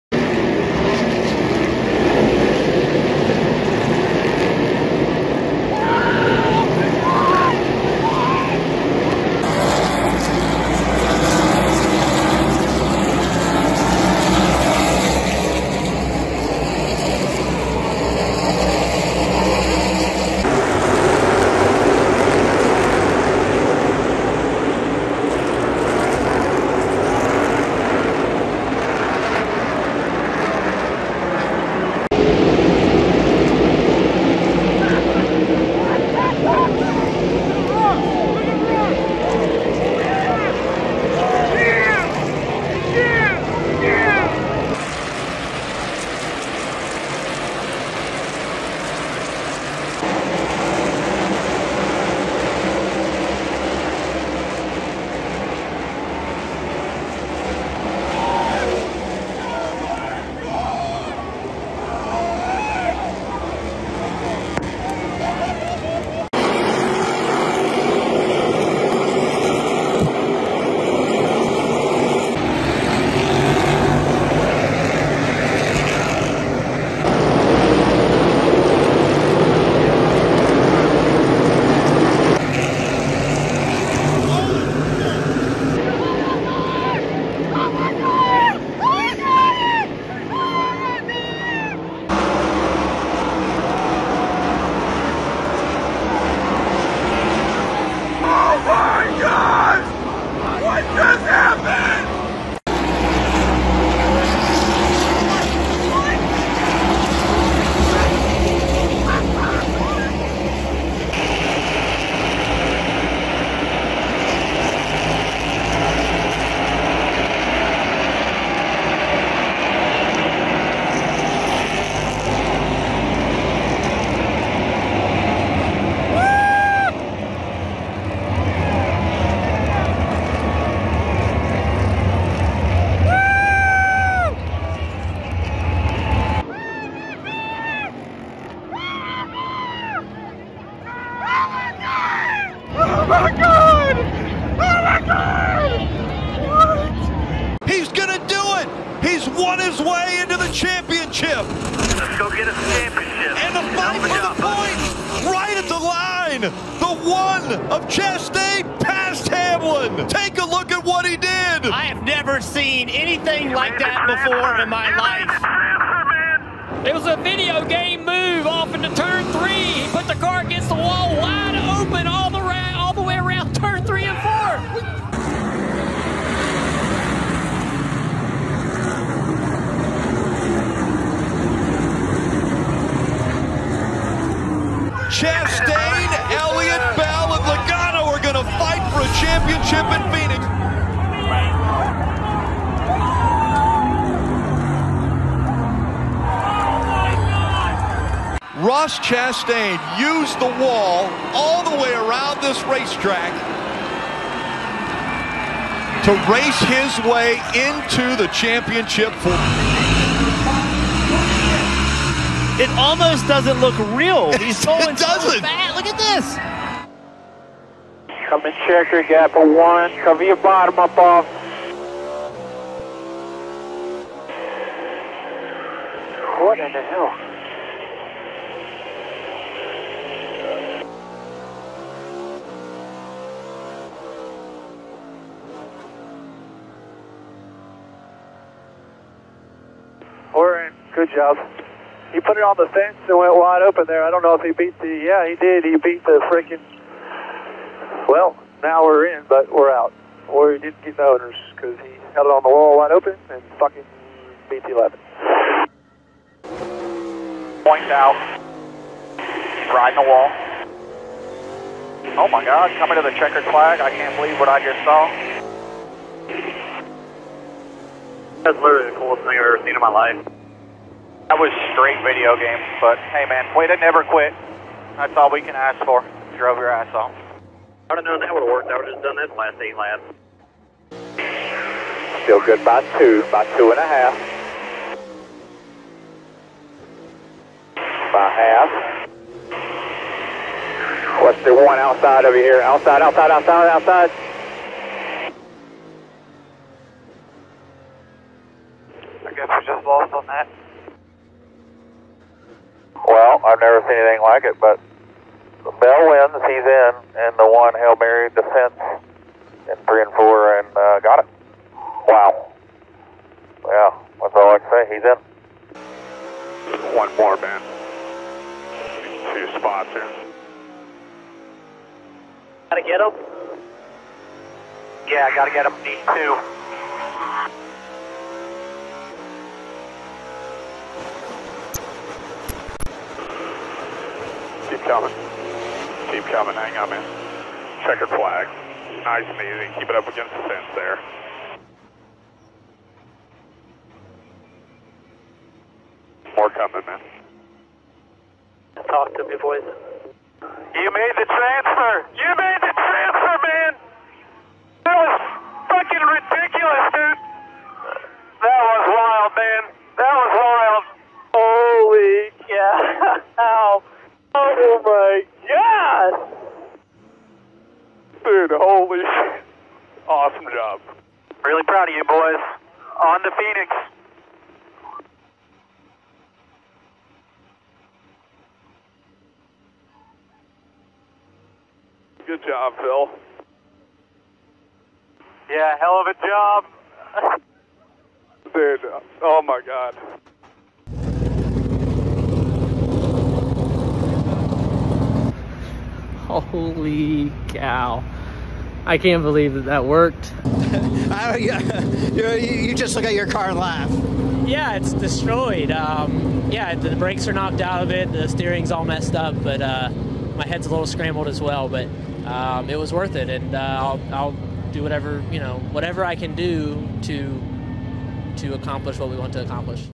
Every Unique Angle of Ross Chastain's VIDEO GAME Move In REAL LIFE!
Ross Chastain needed two more points (positions) with half a lap to go in Sunday's Martinsville elimination race. Going into turn 3, he point's his car into the outside wall and goes FULL THROTTLE AGAINST the wall to gain FIVE POSITIONS, break the Pole and Cup Car Track record to advance into the Championship 4 Finale race at Phoenix! A move previously only done successfully on video games and racing simulators! The ballsy move eliminates his rival Denny Hamlin leaving NASCAR drivers, announcers, and fans are in shock! A truly innovative never-been-done successfully before maneuver in the sport's 70+ year history!

Timestamps:
0:00 Grandstand Fan Videos
2:27 Commentator's Box (TV)
2:55 #1 Team Reactions
3:29 Joey Logano On Board
3:36 Chase Elliott On Board + Team Audio
4:16 Chase Briscoe On Board + Team Audio
4:40 Denny Hamlin On Board + Team Audio
5:15 Ross Chastain On Board + Team Audio
6:31 Ross Chastain Interview / Reactions